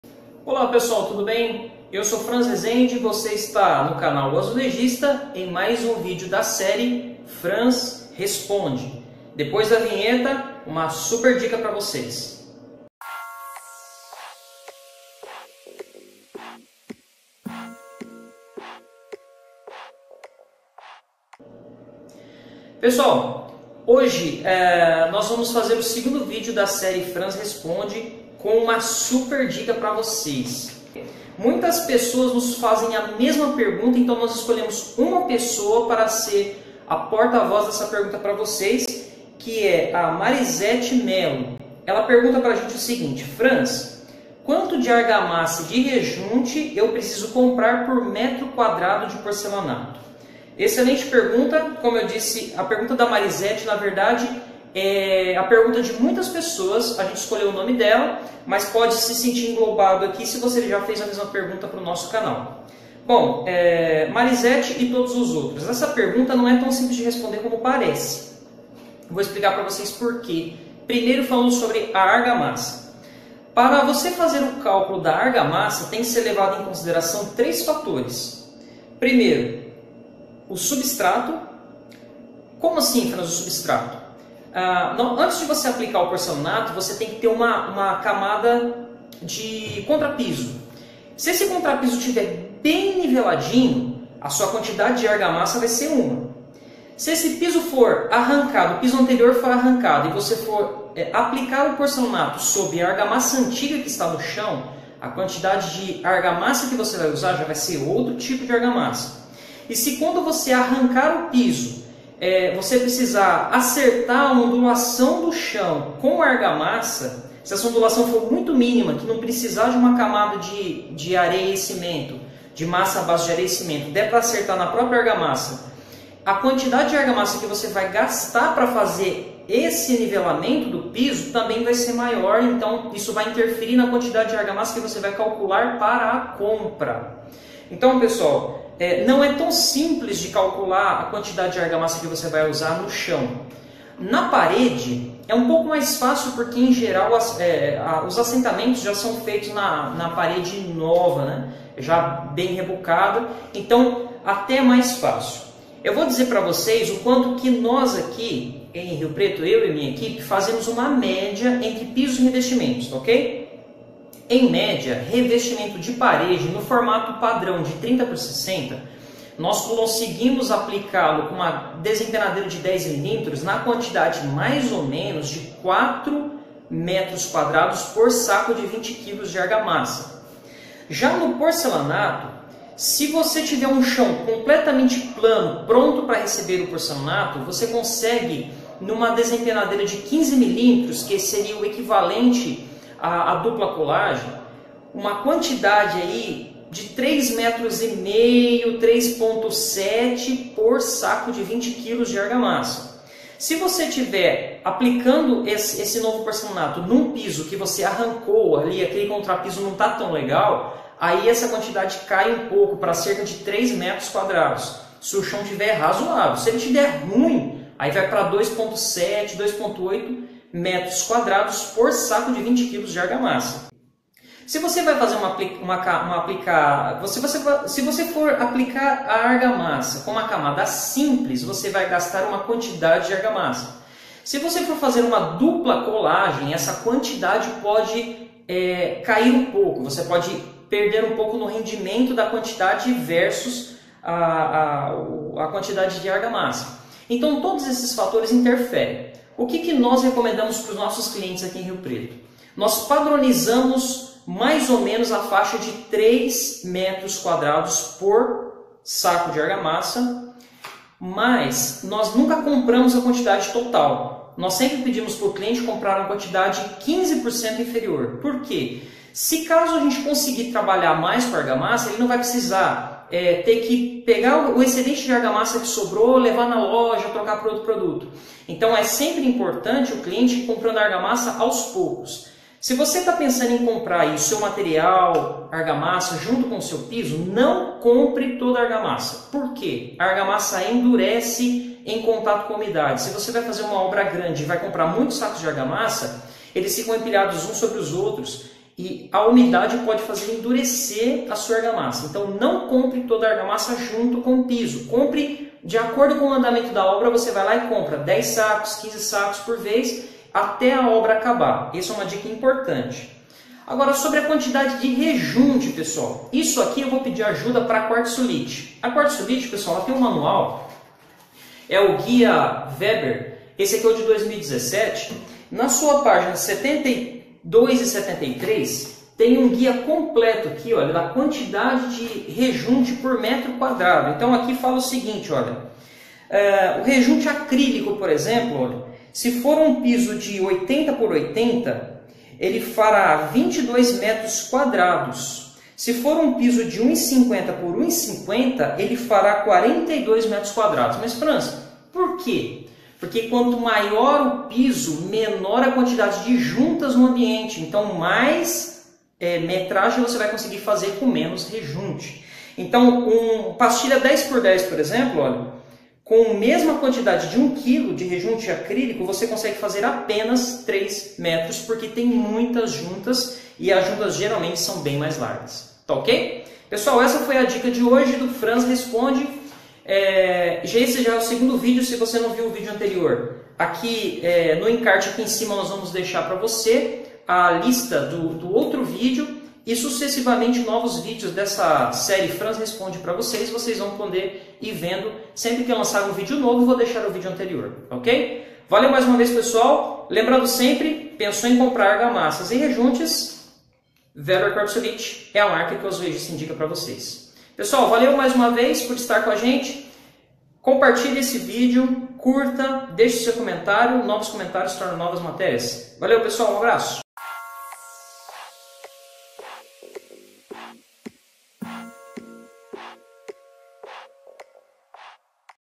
Como calcular a quantidade de argamassa para piso?
Este é o segundo video da série Franz Responde.
Como calcular a quantidade de argamassa e rejunte por m2.